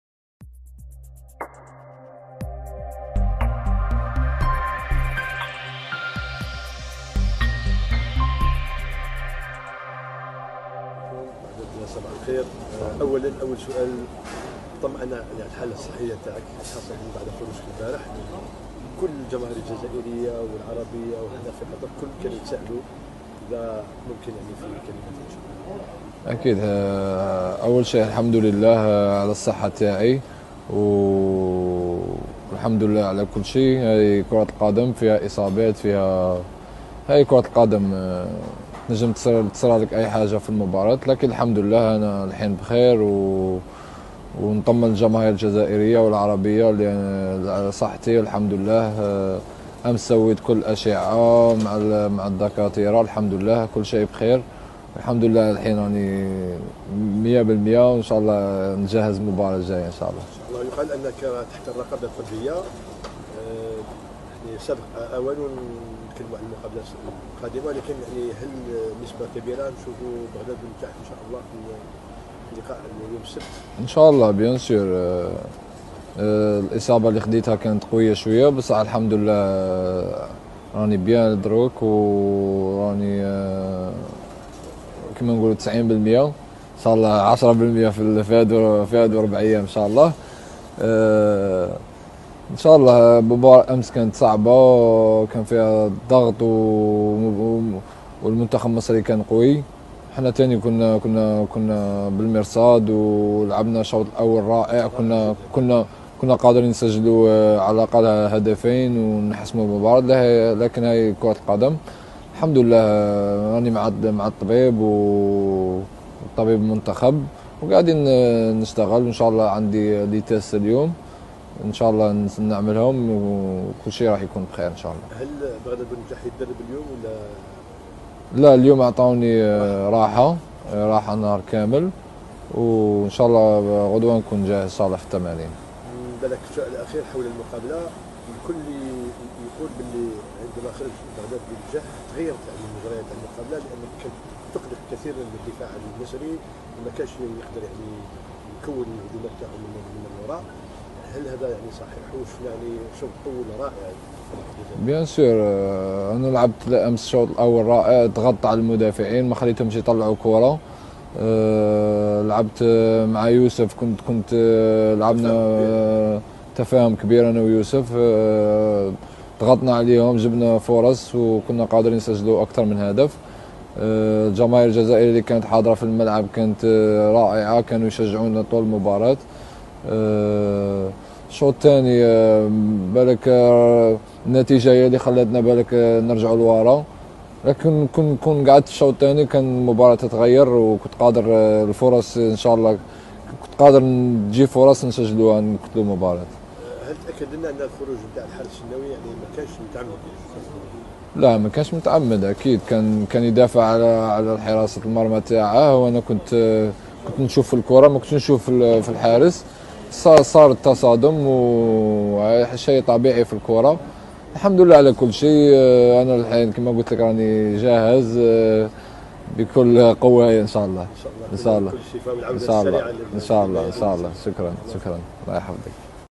بغداد بونجاح يطمئن الجمهور الجزائري و يبعت برسالة قوية ضد المغرب
أخبار رياضية

المنتخب الجزائري